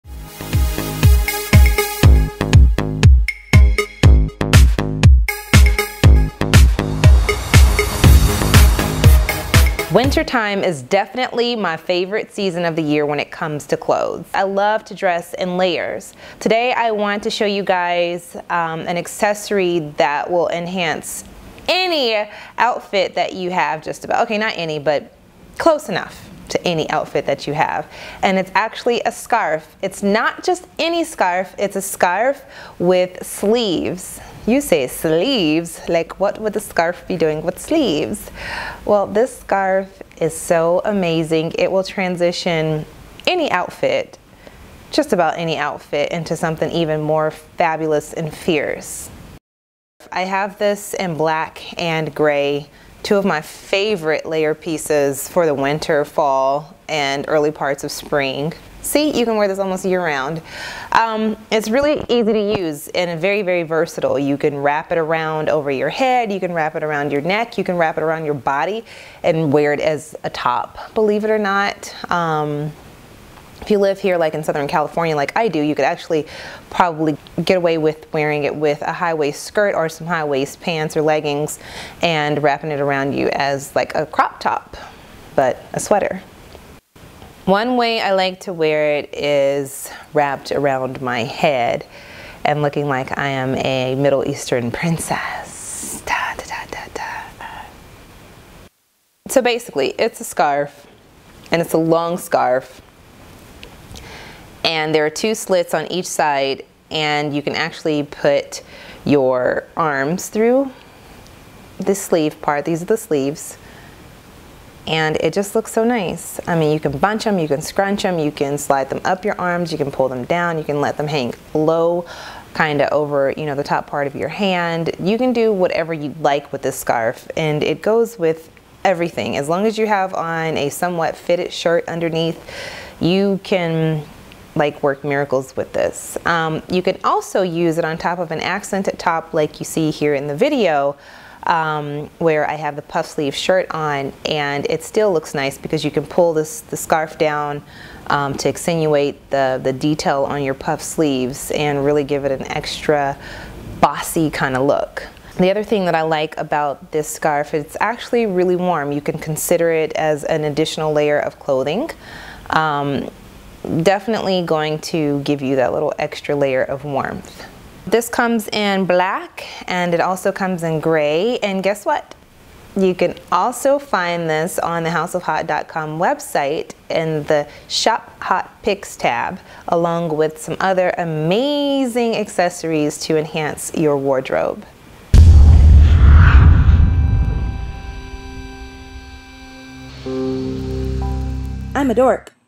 Amazing Scarf with Sleeves!
If you like layers, you are going to LOVE this scarf. It is very versatile, chic, unique, warm and most of all BUDGET FRIENDLY! This amazing knit sweater scarf is so soft and thick. It's the perfect gift!

Shop the Look:
Scarf in Black (Also available in grey): $32 House of Haute

How to Wear a Sweater Scarf:
(A comprehensive demonstration video)